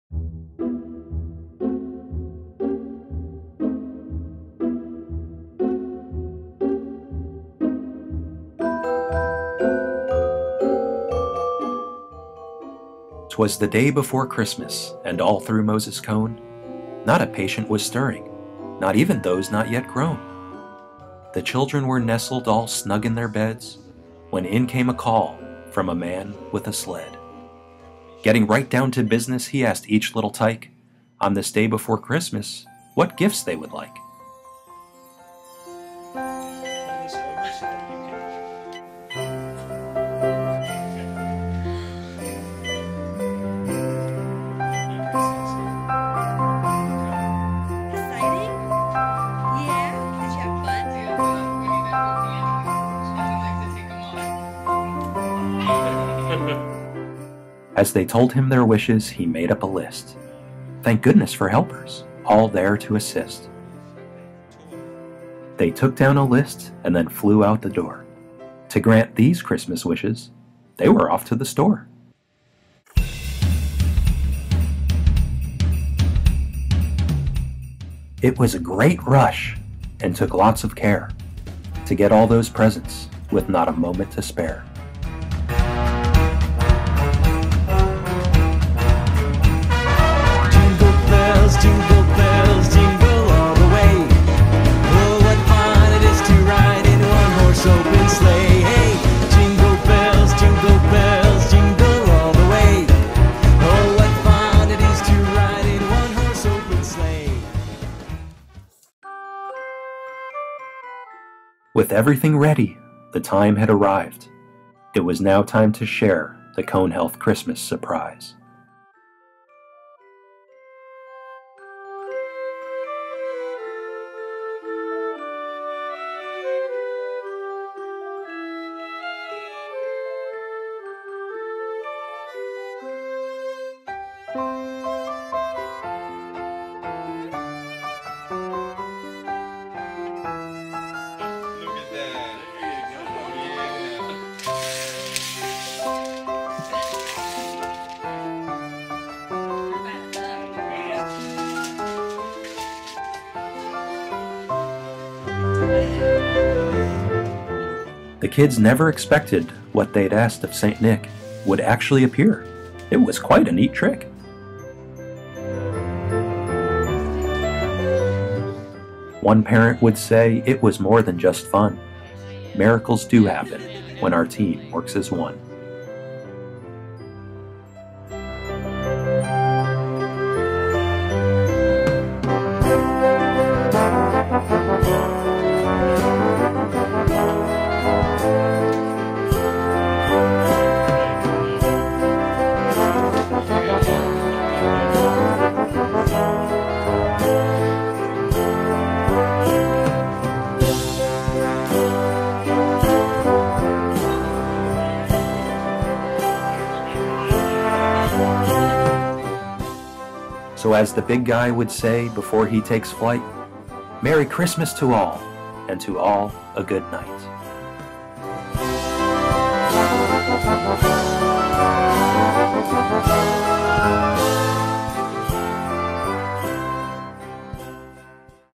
A Cone Christmas
In this video, a special visitor traveled from up North to bring gifts of hope, love and fun surprises to a bunch of special little people at Moses Cone Hospital.  The Cone Health Office of Patient Experience captured the entire journey.